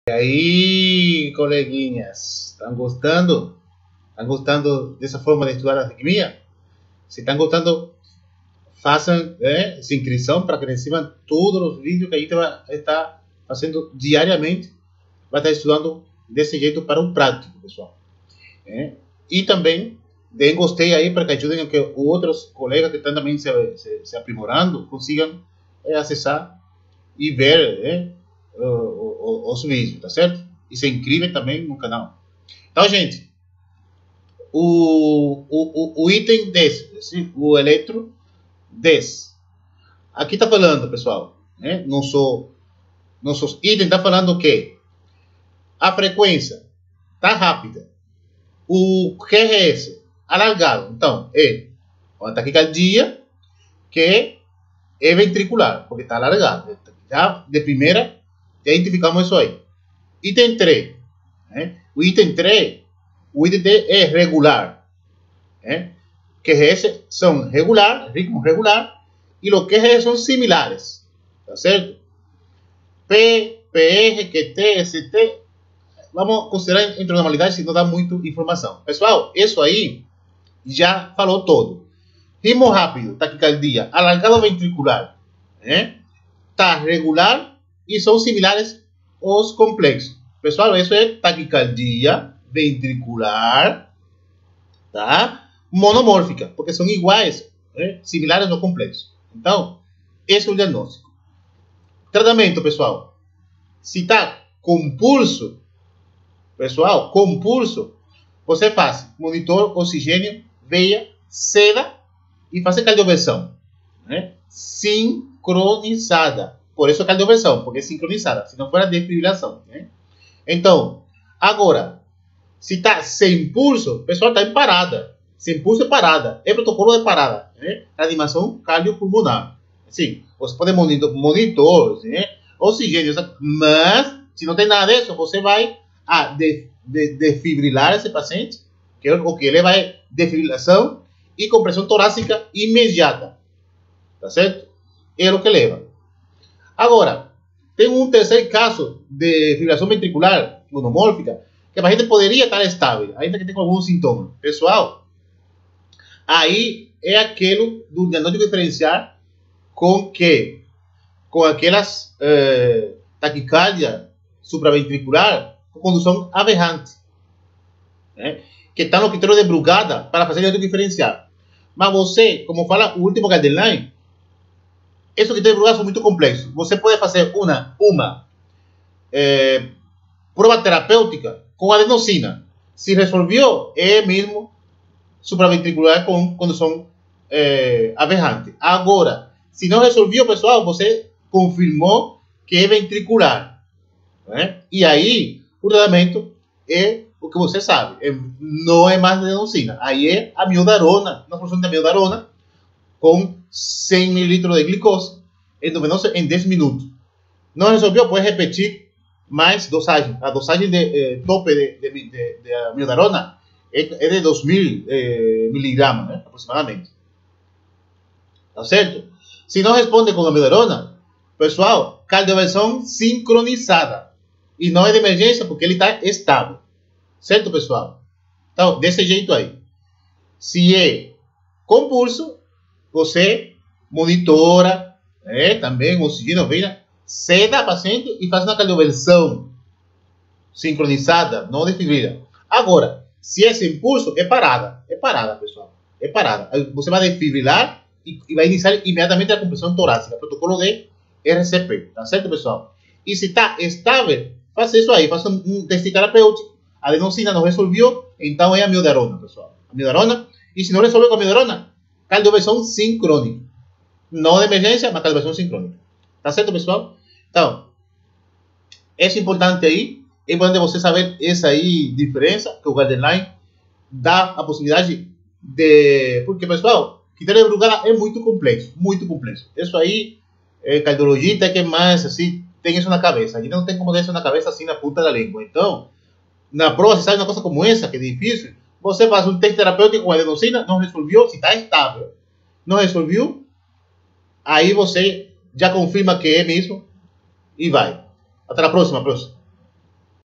25 Pratico Revalida, IDENTIFICANDO E TRATANDO ARRITMIAS (parte 5)
Continuando o treino com método eficaz e fácil, pra lembrar as Arritmias pra o pratico Revalida.